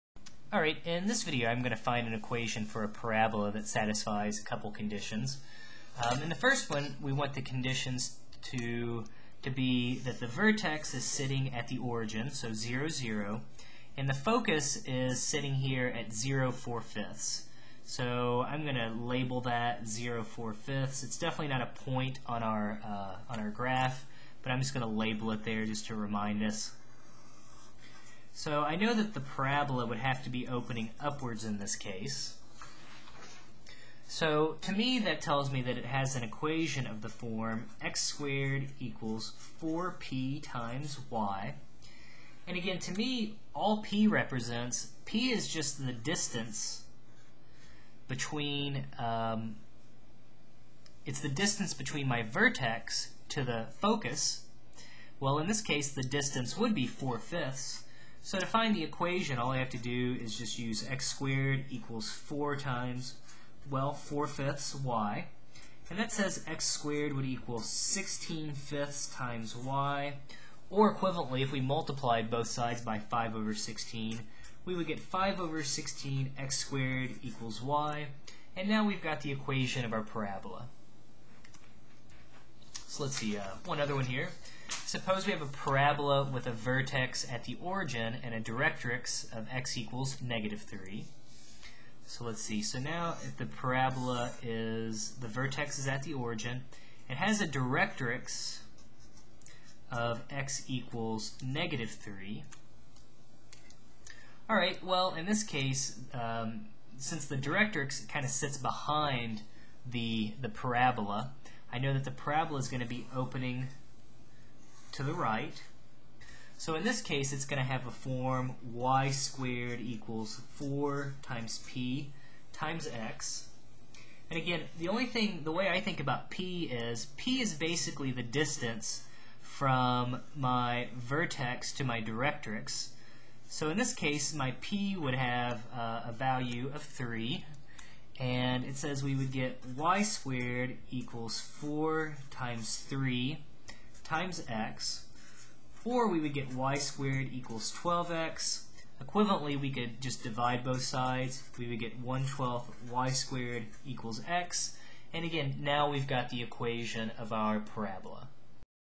Conic Sections: Parabolas, Part 4 (Focus and Directrix)
You da real mvps!   $1 per month helps!! Conic Sections: Parabolas, Part 4 (Focus and Directrix).  In this video, I find the equation for a parabola given the vertex and given the focus and/or directrix.